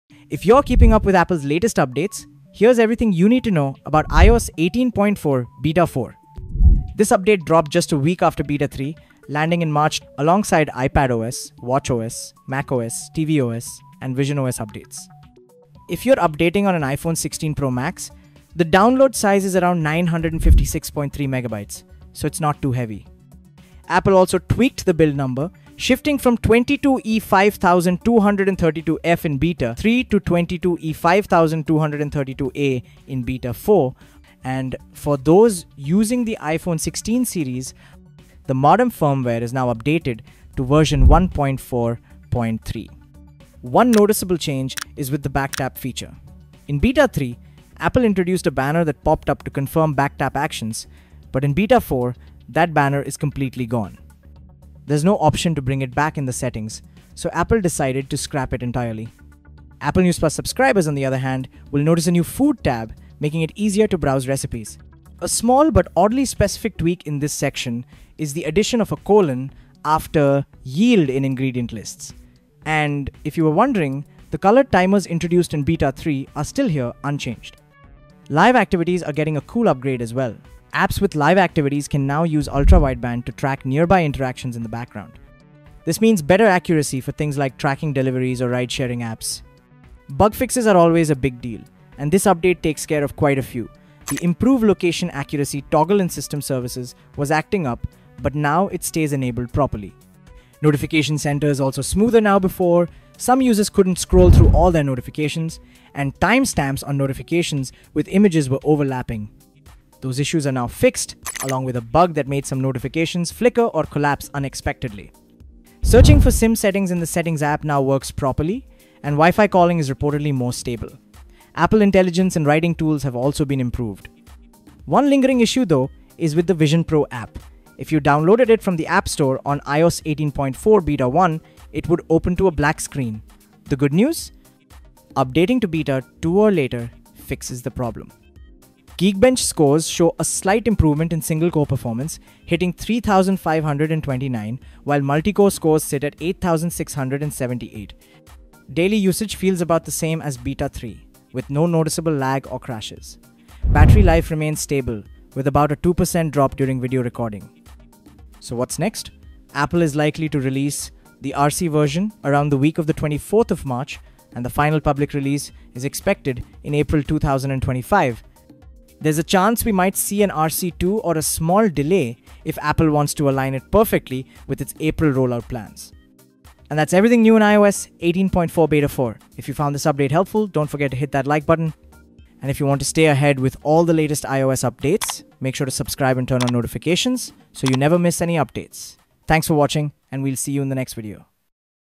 iOS 18.4 Beta 4 Hands-On – Apple Just Fixed Everything!
In this video, we take a deep dive into iOS 18.4 Beta 4, the latest update from Apple that dropped in March 2025. From subtle UI changes to important bug fixes, we cover everything you need to know! Whether you're rocking the iPhone 16 Pro Max or just following the latest iOS leaks, you'll find new tweaks to Live Activities, Back Tap, Apple News+, and even modem firmware updates. Plus, Apple has improved location services, notification center behavior, and Wi-Fi calling stability. ⚙️📱

🔗 Follow for more tech updates!